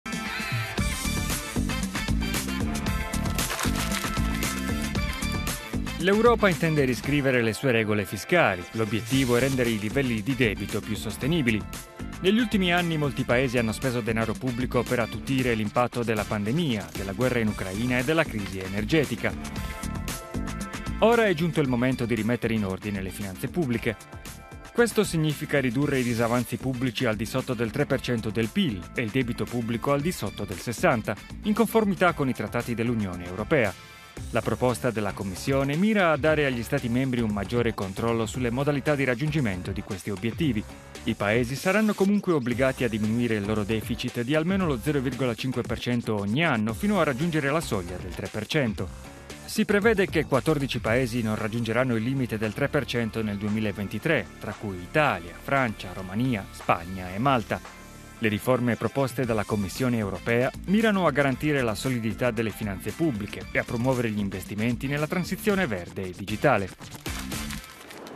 Ue: quale sarà l'impatto delle nuove regole fiscali?
La Commissione europea ha presentato una proposta di riforma delle regole fiscali dell'Unione europea. Dopo l'impennata del debito pubblico causata dalla pandemia, l'Ue intende rimettere in ordine le proprie finanze pubbliche